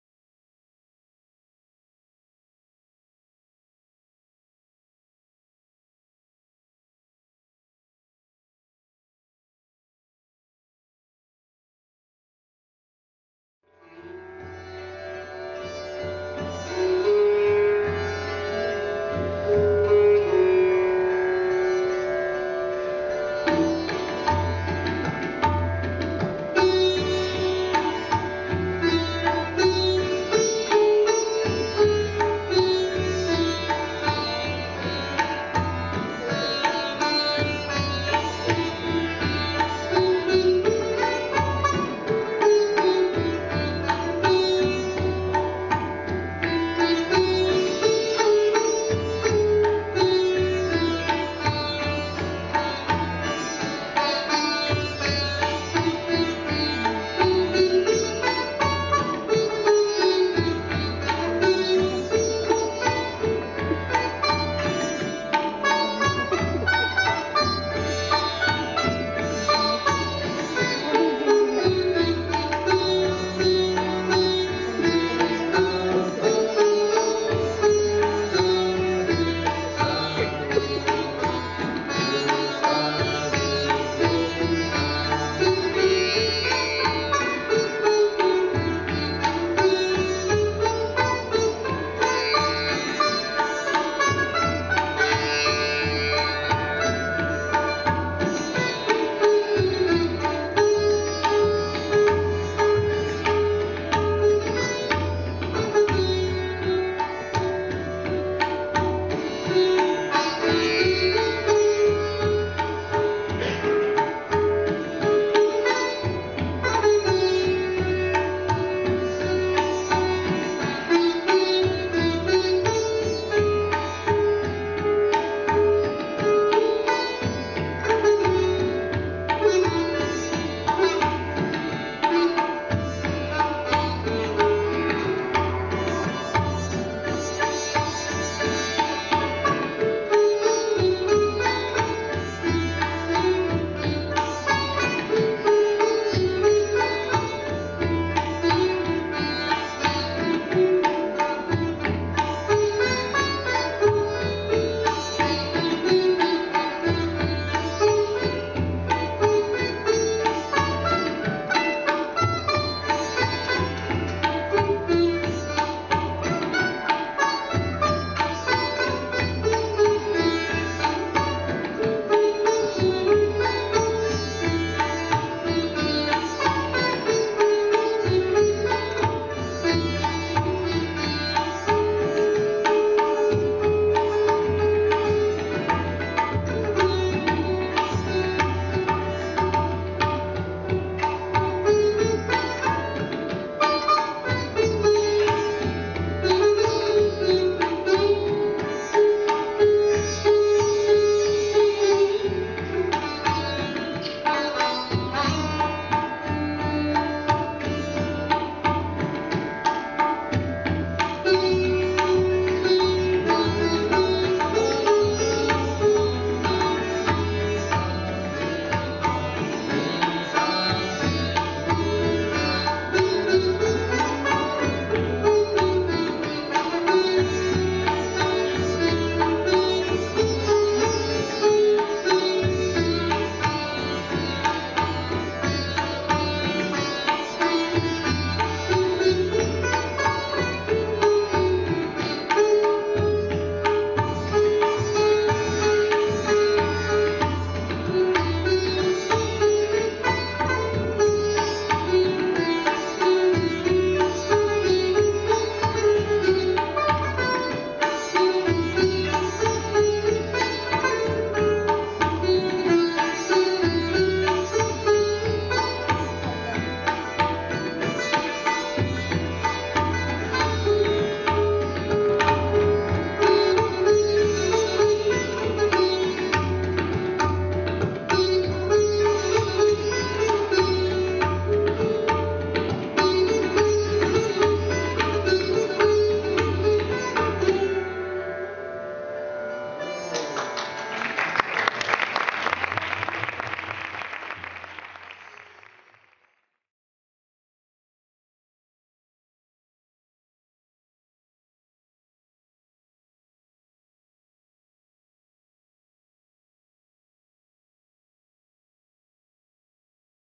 Óliver Cuello y Federico Morales
Óliver Cuello ( sitar ) y Federico Morales ( tabla )
 Auditorio de La Alberca ( Murcia )...03/06/2011